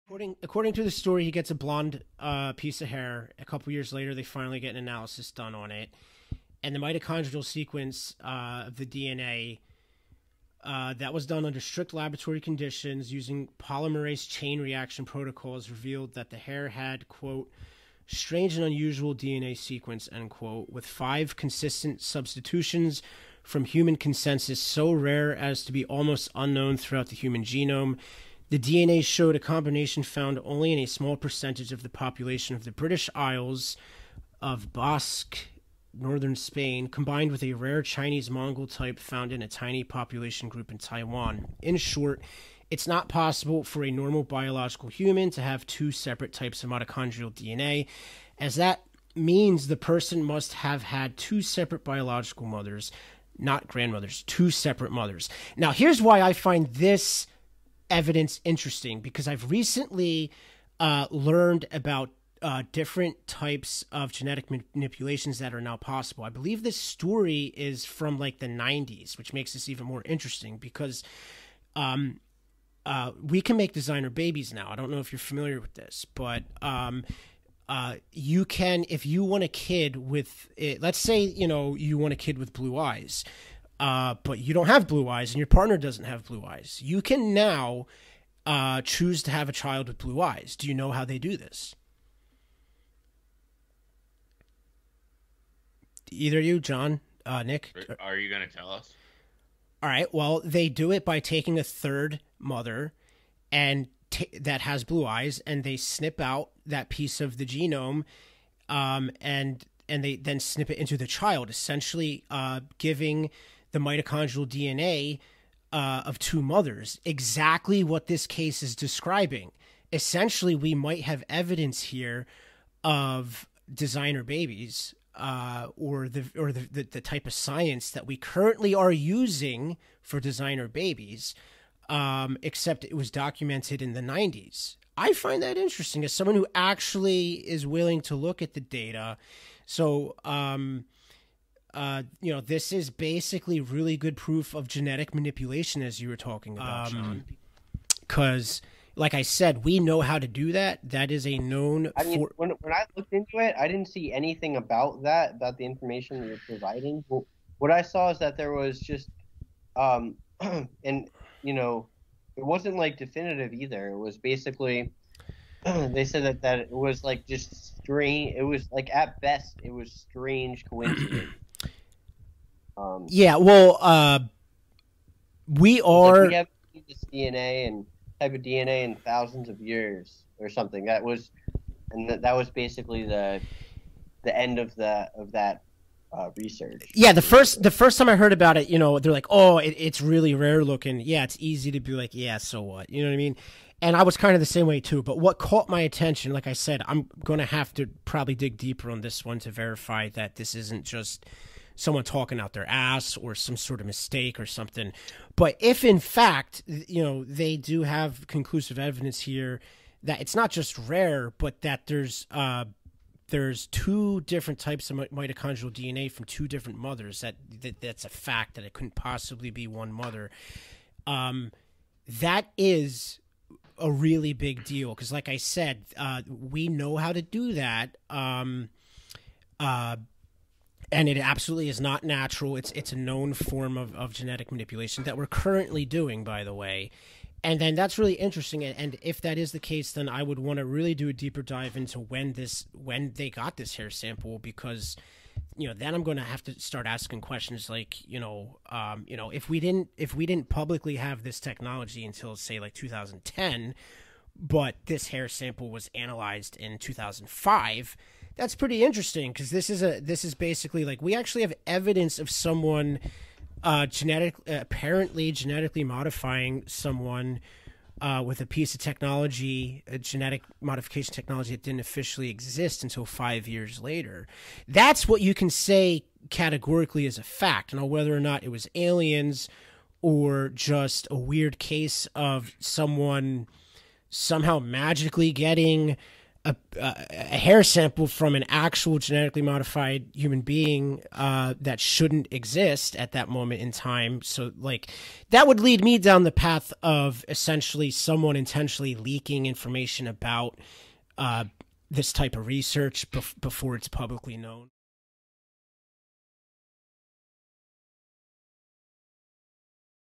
Genetic Analysis of the "Alien" Hair Sample
The hair sample in question was reportedly obtained in 1992, analyzed in 1999 and a book was published in 2005 discussing the investigation.

I've only read a synopsis of the full story, but bought the book and skipped straight to the gene analysis because I realized it sounded like technology we now have today.

What I find interesting about the analysis is that it shows 2 different mitochondrial DNA and there's high confidence that it's not contaminated. This potentially indicates genes from 2 mothers. We now know how to take mitochondrial DNA from one woman and edit it into a baby's DNA to create offspring with DNA from 2 mothers. This can be done to target specific genes to prevent disease or to choose hair/eye color that is otherwise impossible (designer babies.) Not only were there odd matches of sequences that are super rare, but there was mitochondrial DNA of two kinds. Even more interesting, there were two deleted genes for the CCR5 protein which provides immunity for AID's and resistance to smallpox. Basically, it has all the signs of a designer baby.

Even in 1999 the biochemist working on the hair sample speculated that it could be from genetic engineering. I did some digging and found one source that claims this was first done in the 90's but was banned.